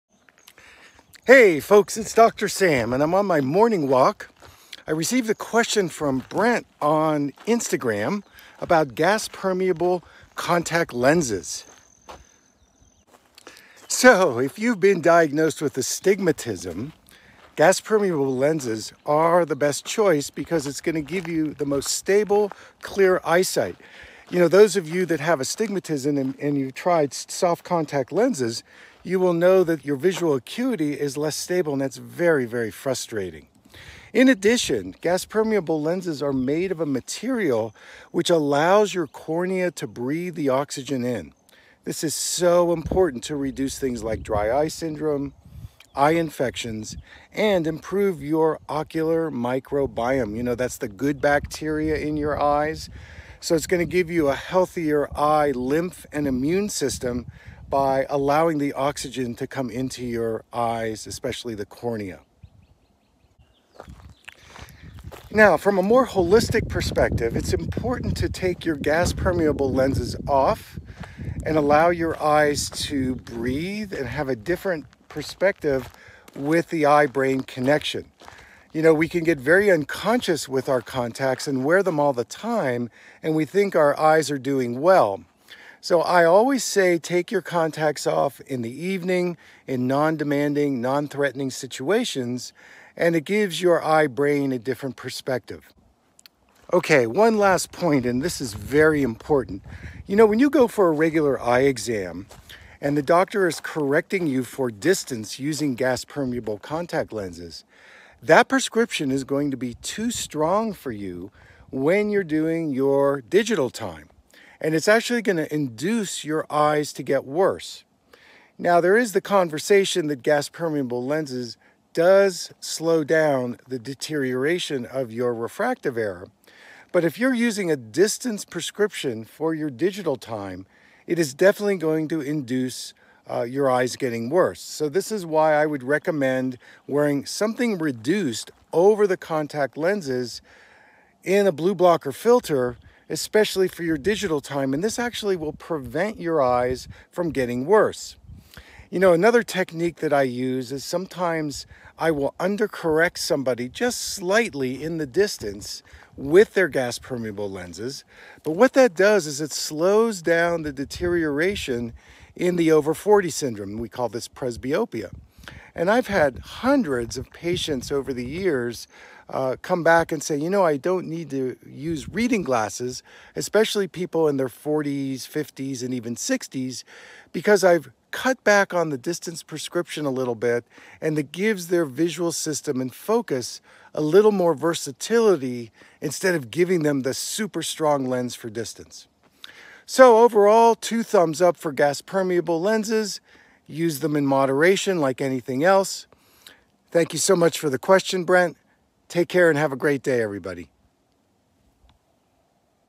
Gas permeable contact lenses
Dr. Sam answers a question about the use of gas permeable contact lenses.